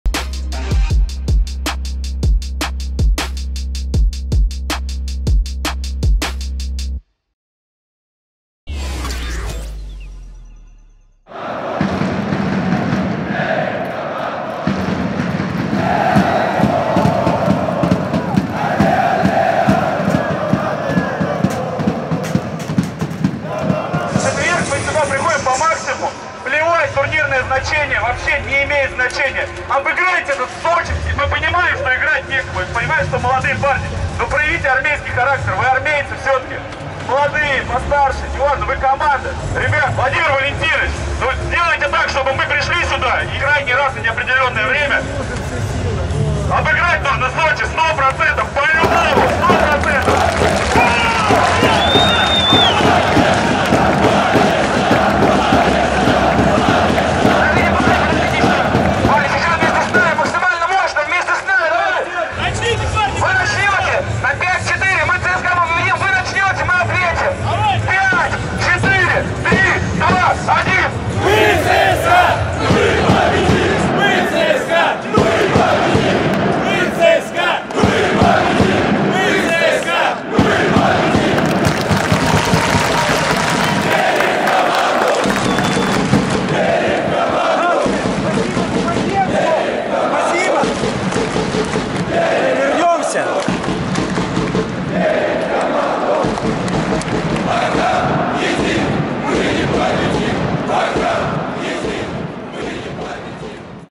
Разговор фанатов и команды после матча
🎥 После неудачной игры команда подошла поблагодарить фанатов, а у главного тренера получился душевный разговор с нашим активом